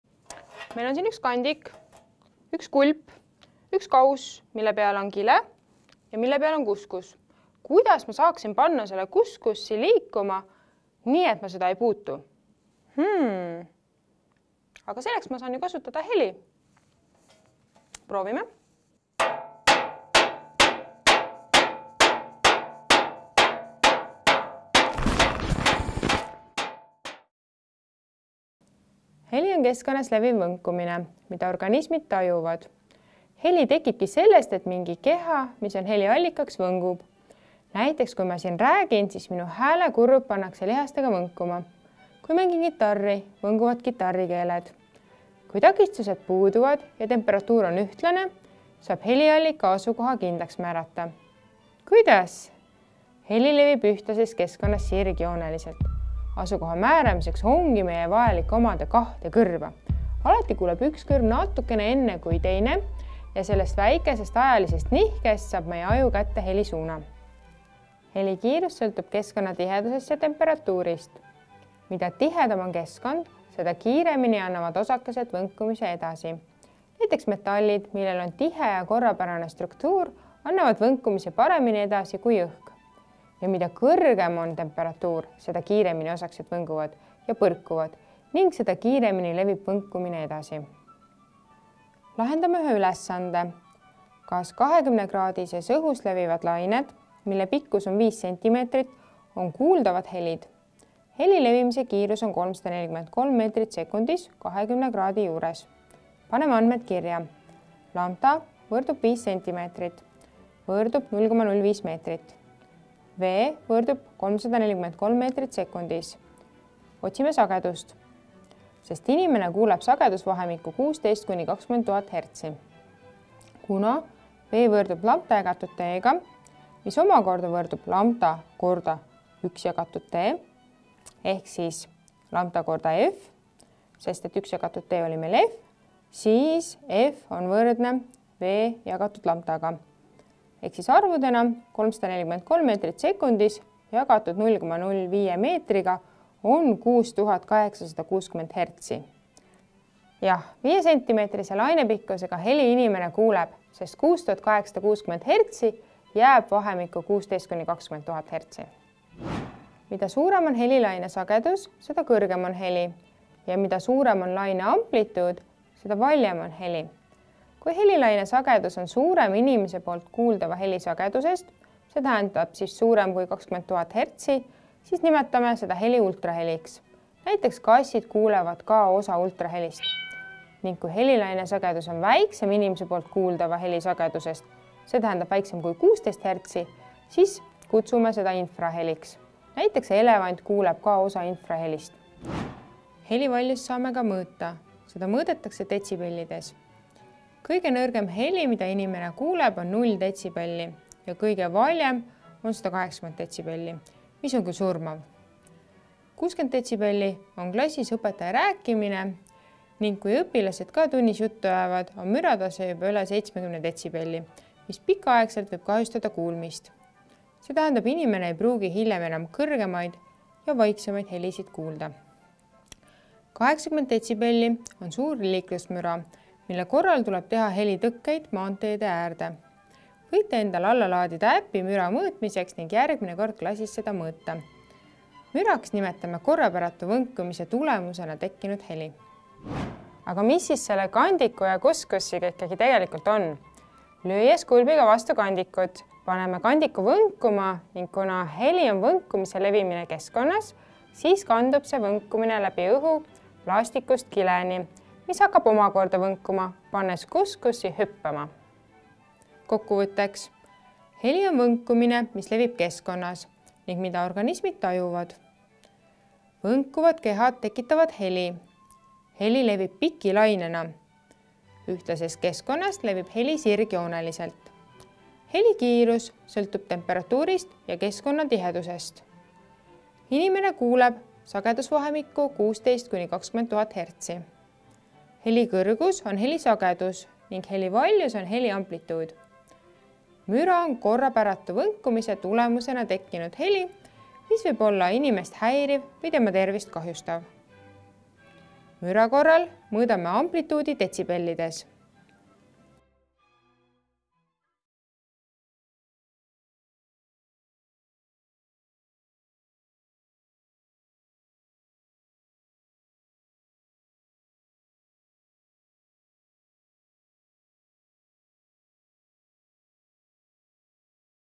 Heli | VIDEOÕPS Füüsika. Mehaanika #11
Mehaanika seeria 11. videos tutvustab Aune heli mõistet ja selle seost võnkumise ning lainetega. Käsitletakse mõisteid: sagedus, ultraheli, infraheli, detsibell, müra.

Video on mõeldud kasutamiseks põhikooli III kooliastme füüsika õppes, kus teemaks on mehaanika.

Sisukord:
00:00 Sissejuhatus
00:29 Heli mõiste
00:45 Heliallika asukoha määramine
01:08 Heli kiirus
01:32 Arvutusülesanne
02:30 Inimese kuuldav sagedusvahemik
02:40 Sageduse ja amplituudi seos helitugevusega
02:48 Ultraheli
03:00 Infraheli
03:13 Detsibellid
03:53 Müra
03:50 Kandiku ja kuskusi katse seletus
04:19 Kokkuvõte

Video on valminud heade Eesti ettevõtete toel, Lae End programmi raames.

Video sisu: Aune Nigol, Kristo Siig, Ingrid Rõigas
Kaamera ees: Aune Nigol
Filmimine: Martin Männik
Montaaž: Anders Aruoja
Režissöör ja projektijuht: Kristo Siig

Videole on omistatud CC-BY autoriõiguste litsents, mis võimaldab materjali vabalt taaskasutada, kui viidatakse autorile (MTÜ Videoõps, Praktikal Education OÜ ja SA Enefit Tõukefond).